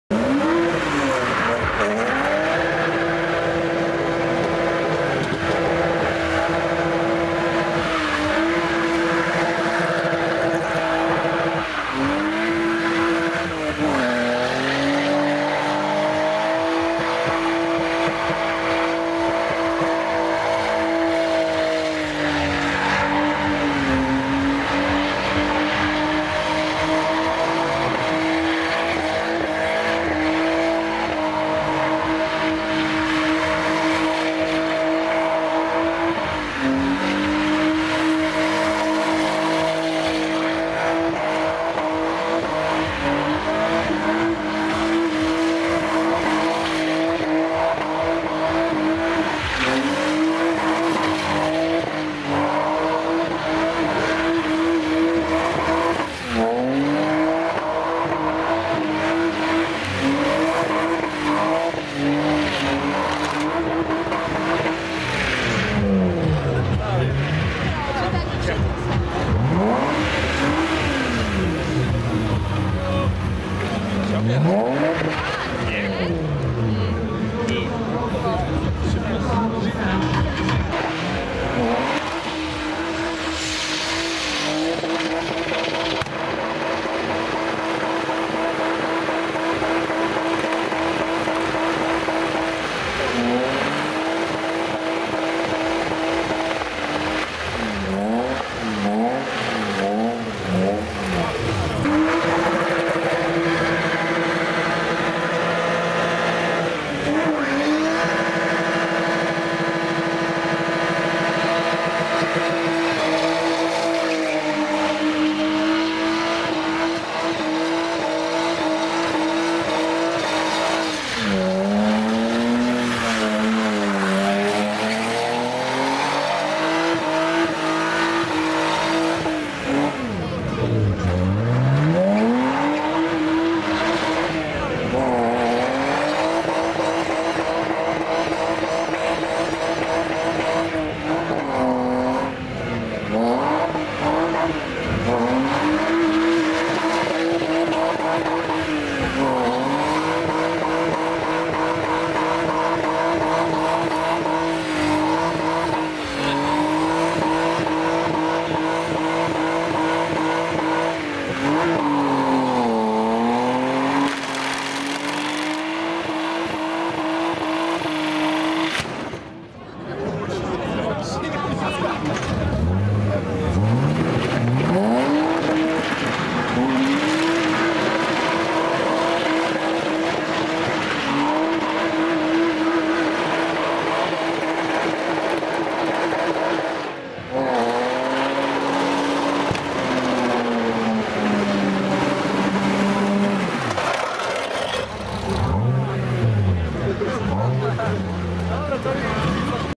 Eliminacje pokazu mocy BMW E36 328 "TOP MEBEL" - Majówka z BMW Toruń 2019
Z problemami ale jak zwykle efektownie i przy dźwiękach pięknej odcinki.